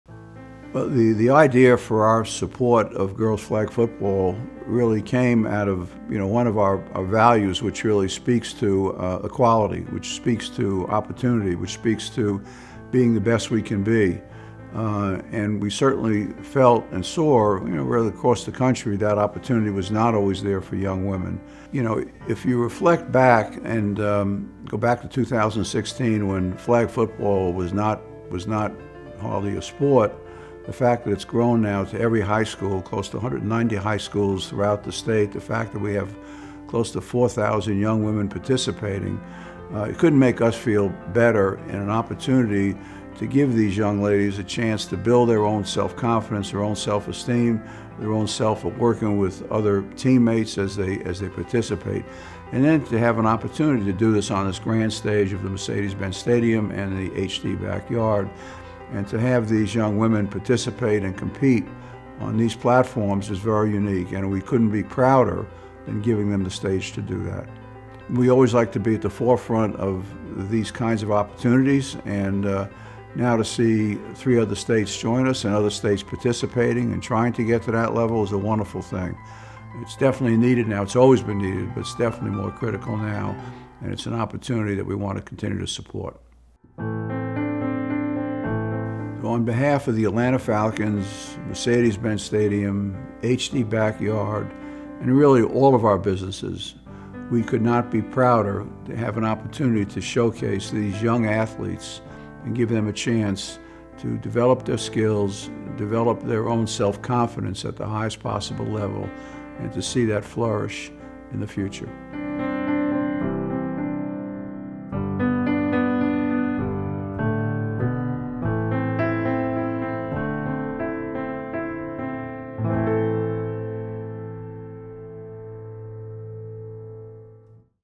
Atlanta Falcons & Mercedes-Benz Stadium announce a new display dedicated to Girls Flag Football
Over the past six years, the Atlanta Falcons and The Arthur M. Blank Family Foundation have worked tirelessly to elevate the sport of girls flag football to be sanctioned across the United States to promote gender equality in sports offered at the high school level. It has become one of the fastest growing sports and other states are following the Falcons’ lead.

On May 11, the Falcons and its home, Mercedes-Benz Stadium will unveil a display dedicated to the trailblazers who pioneered girls flag football and continue to build the sport in the state of Georgia and beyond.